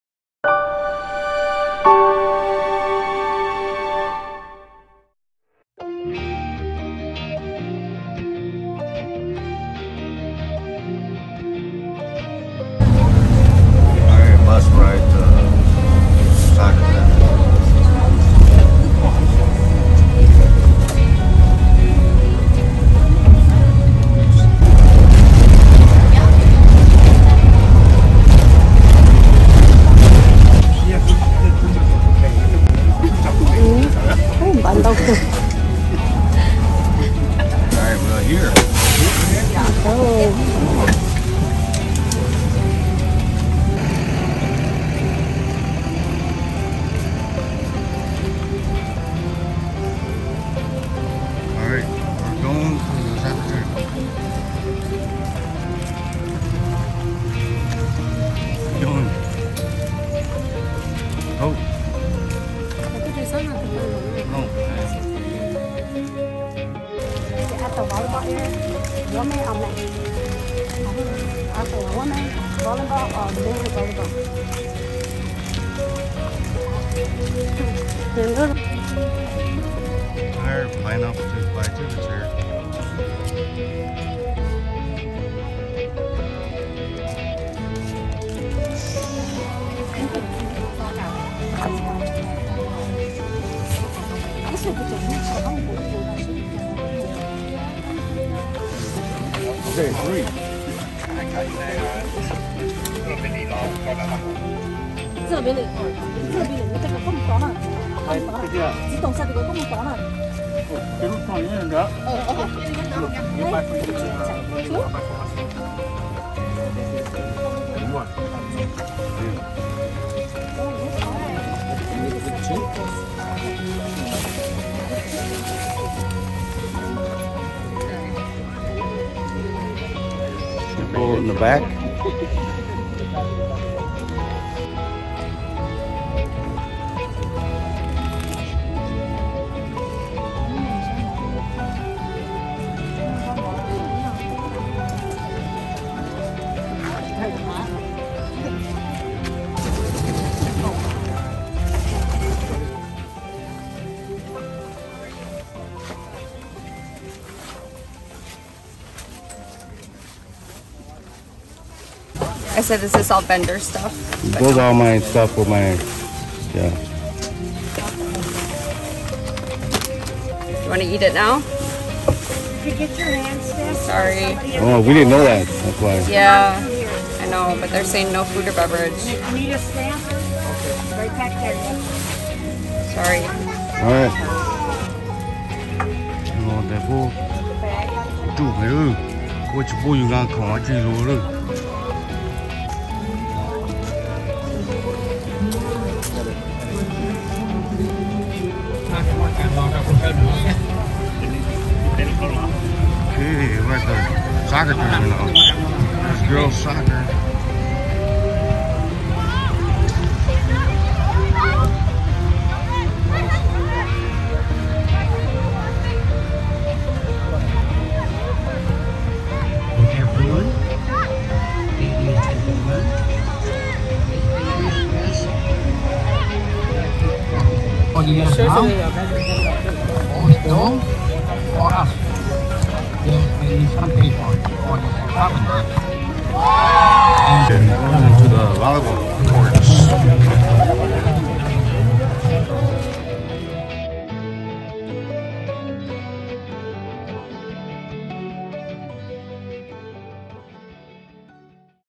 Our Experience at the 2022 Soccer Tournament
Let me walk you through the event if you've never been at the Soccer Tournament before.
It's a great place to meet up with old friends, new friends and family. And you get to watch a lot of sports events that is worth your money. Come take this short journey with me and you'll see!